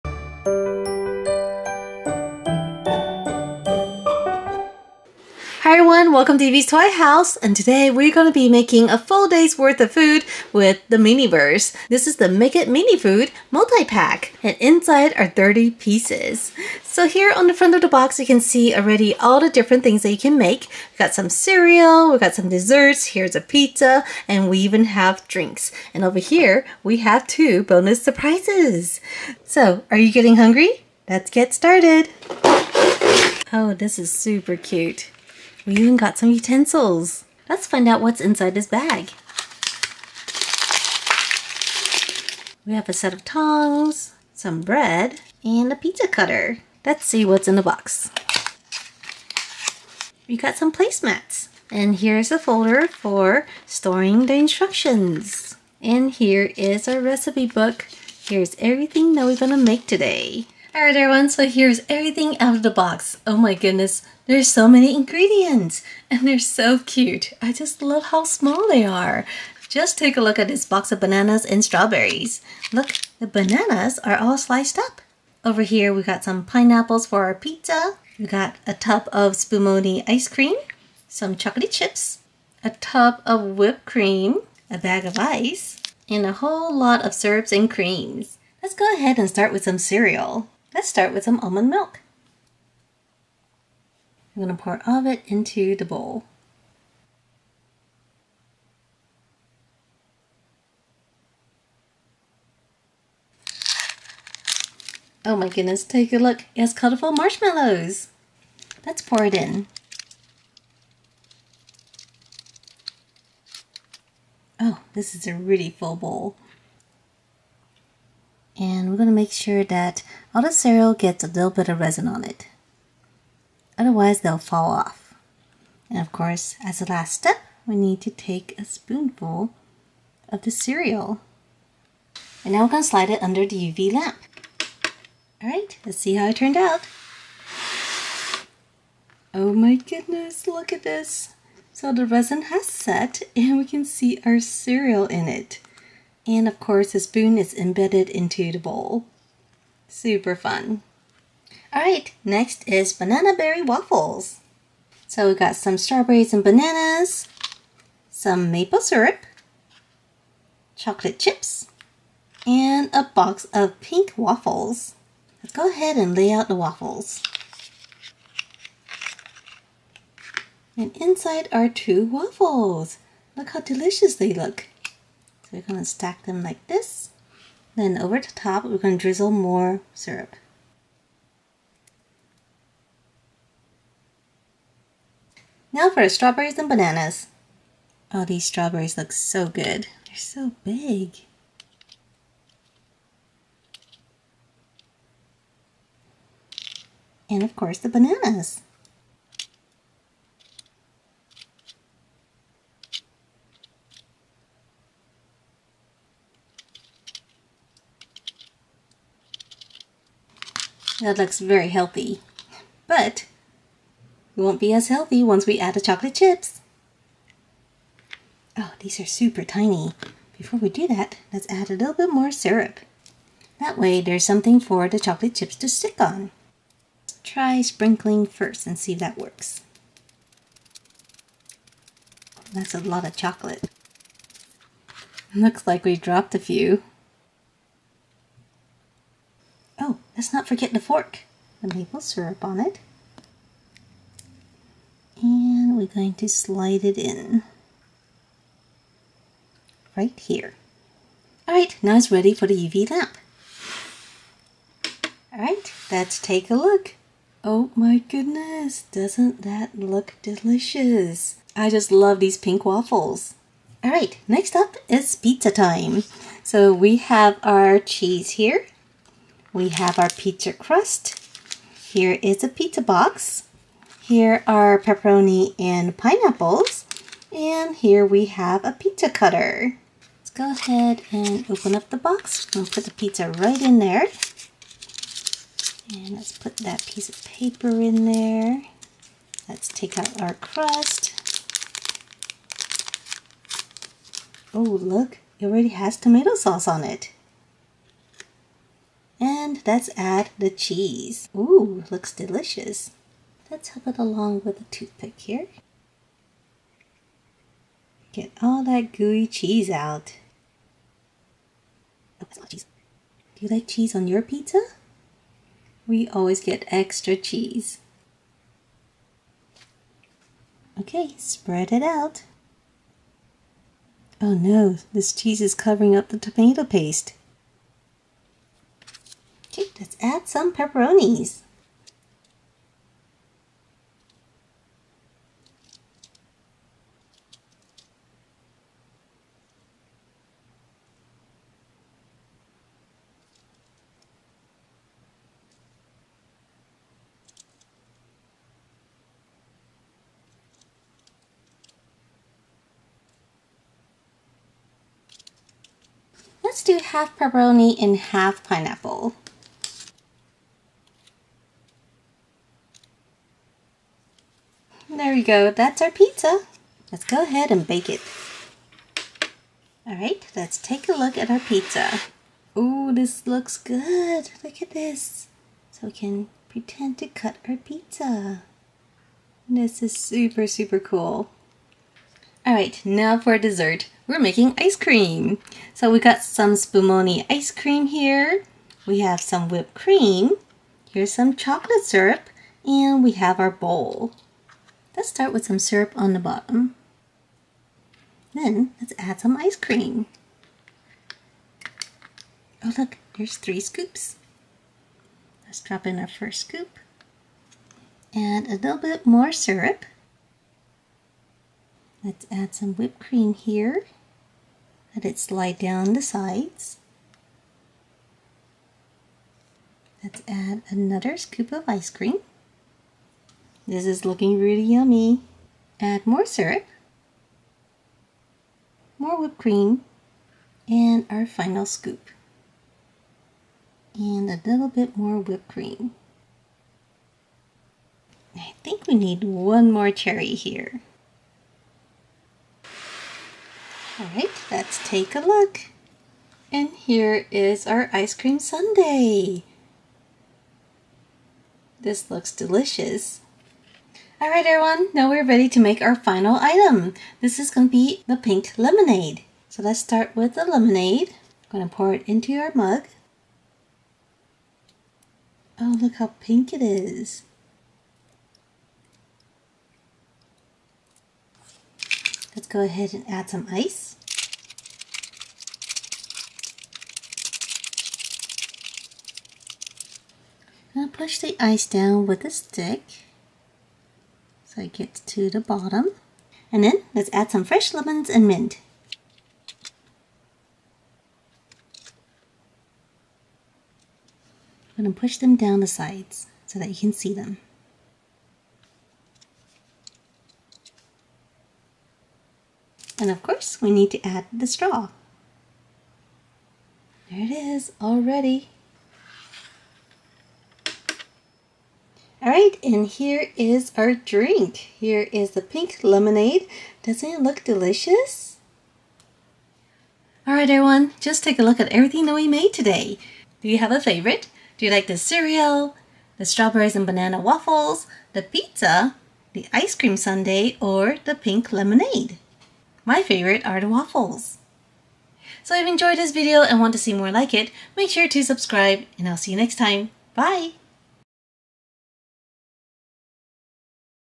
Mini Verse Make it Mini Food Multipack Resin Craft
Let's make a whole day's worth of food with the Miniverse Make it Mini Food Multipack! This fun pack comes with 30 items to make 5 entries. They include main, dessert, snacks and drink!

Make it yourself!
- Make It Mini Kitchen
- Mini Food Diner Serie 2
- Make It Mini Multipack

DIY Custom Supplies